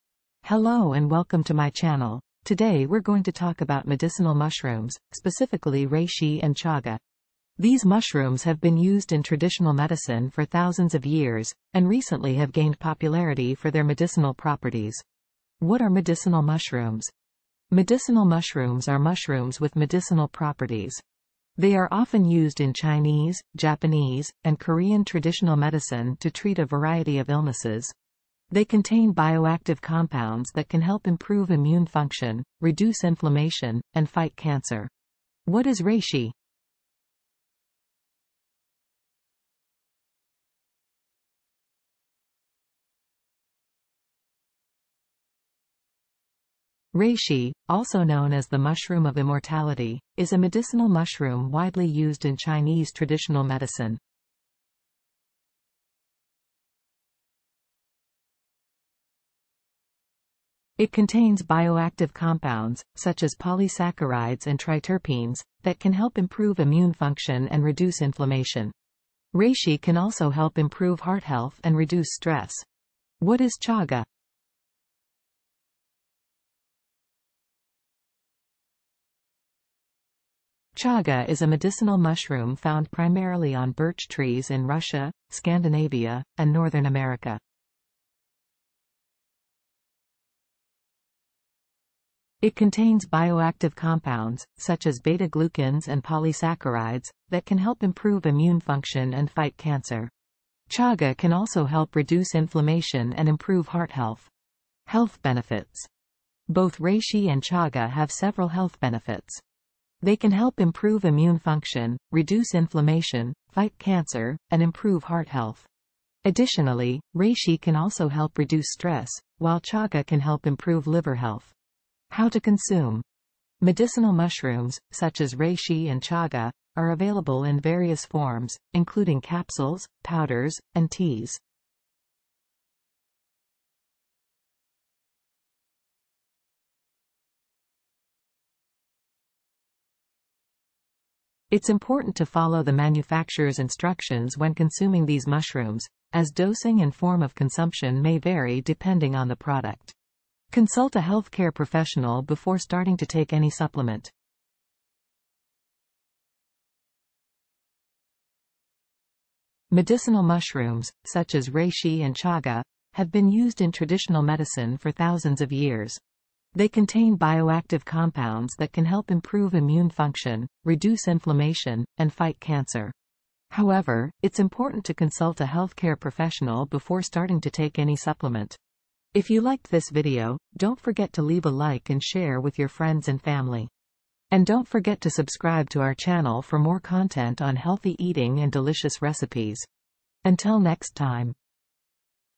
Unlocking the Healing Powers of Reishi and Chaga: The Medicinal Mushrooms That Can Change Your Life!
Hello everyone!
today we're going to talk about medicinal mushrooms, specifically Reishi and Chaga. These mushrooms have been used in traditional medicine for thousands of years, and recently have gained popularity for their medicinal properties.